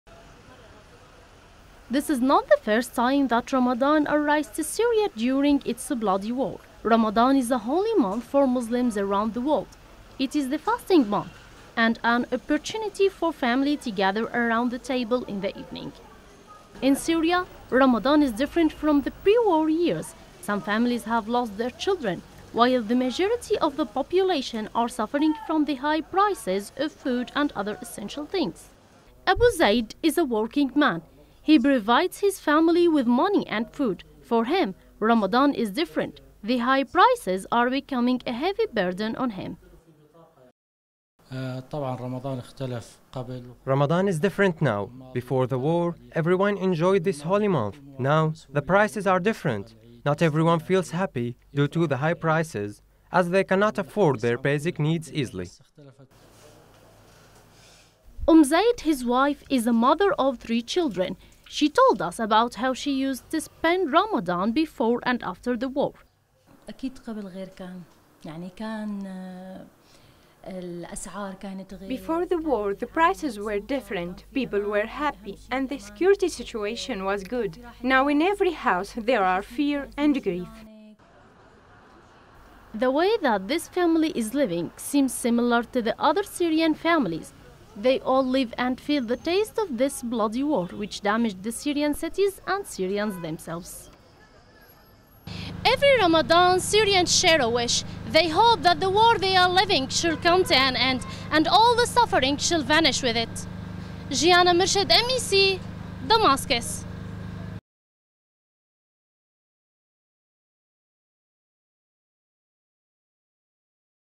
Ramadan in Syria
Ramadan gained a bitter taste due to the war.

This is not the first time that Ramadan arrives to Syria during its bloody war. Ramadan is a holy month for Muslims around the world, it's the fasting month, and an opportunity for family to gather around the table in the evening.